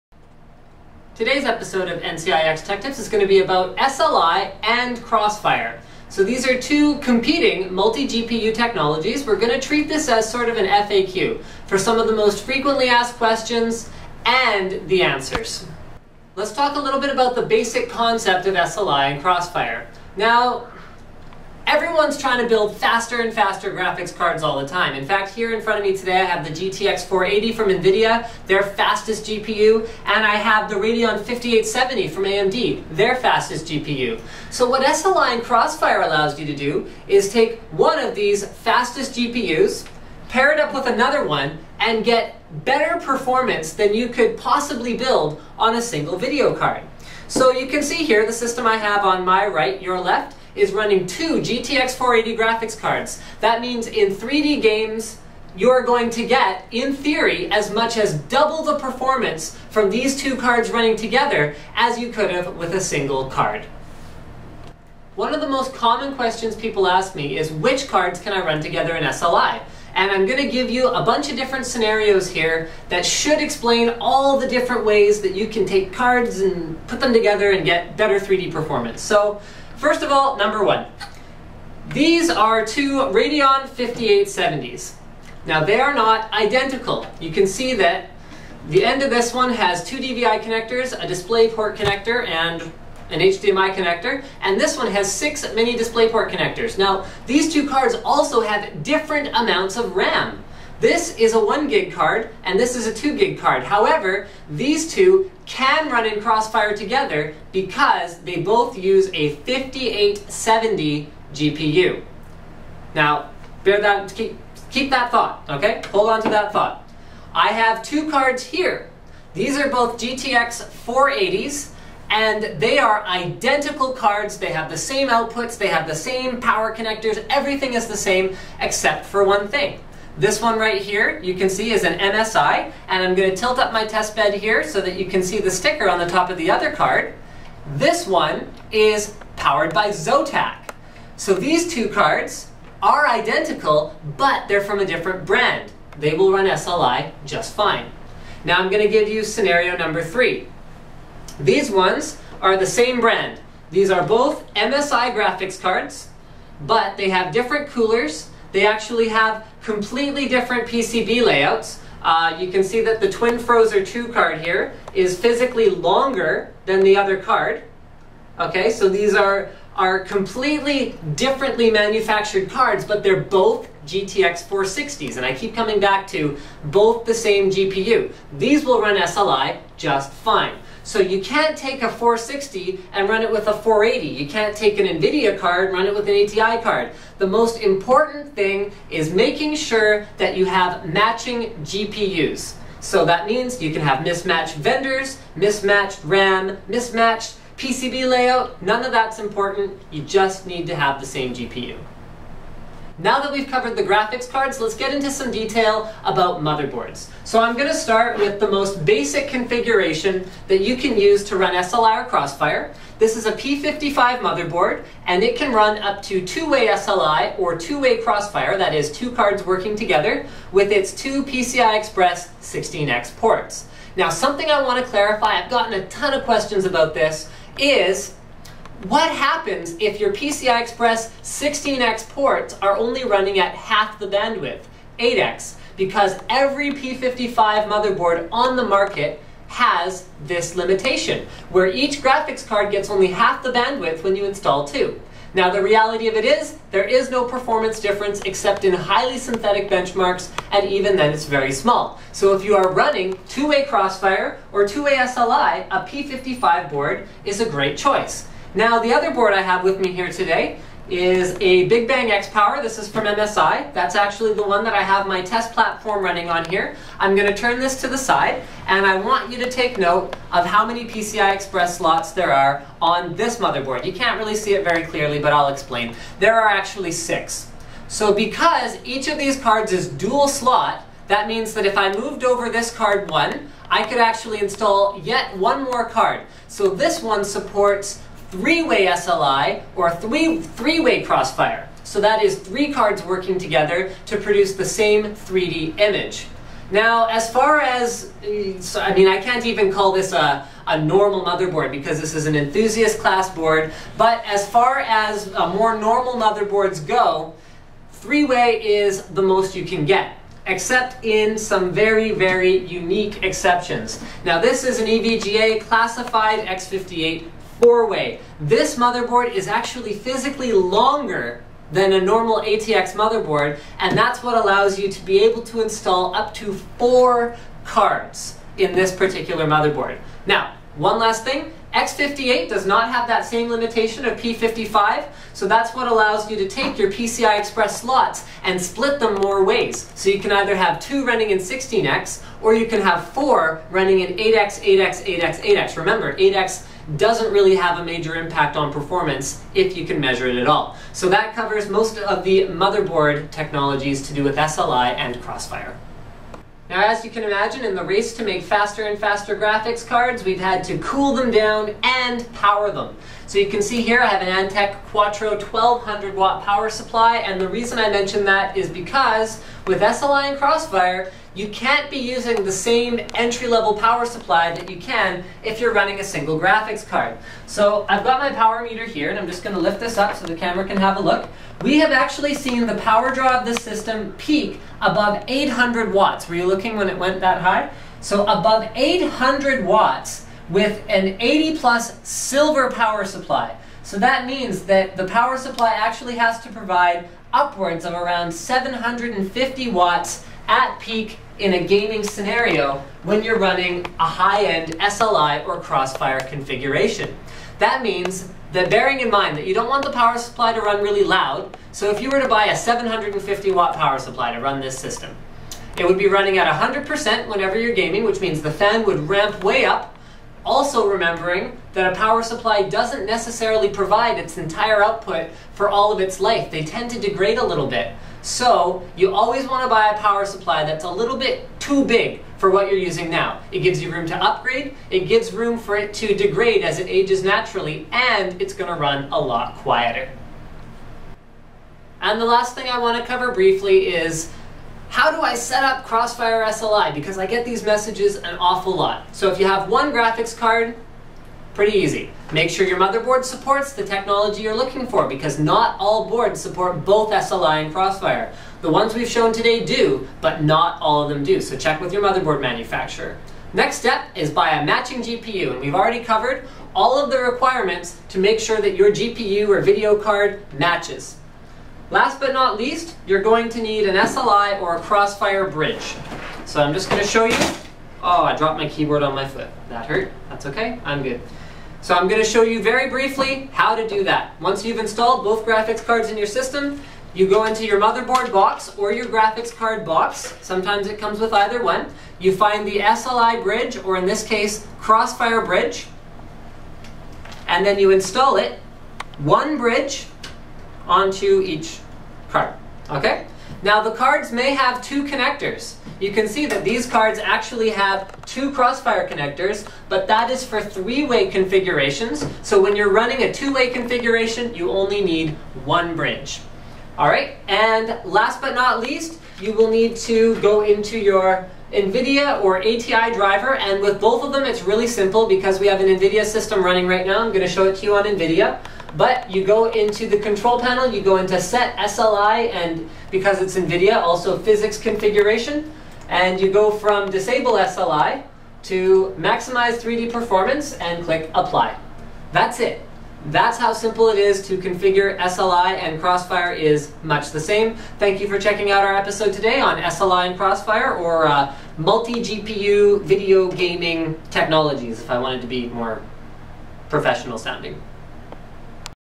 SLI & Crossfire FAQ & Tutorial (NCIX Tech Tips #89)
Today Linus goes over some of the more common questions about these two technologies and helps viewers understand how to make their own PCs SLI and Crossfire ready.